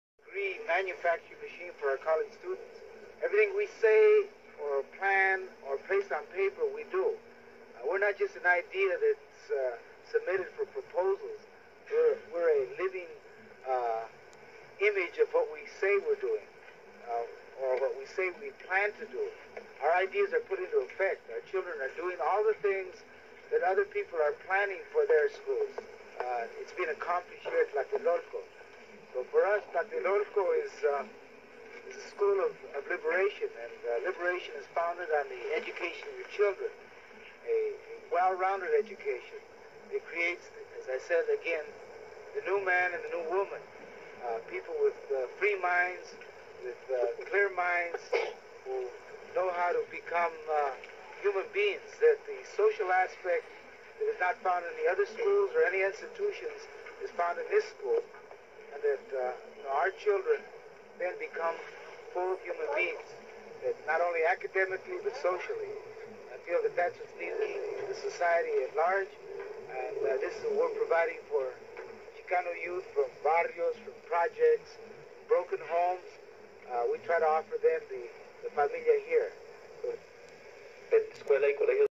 Escuela Tlatelolco education philosophy by Corky Gonzales
Escuela tlatelolco co-founded by The Crusade for justice Denver Colorado 1970 closed 2015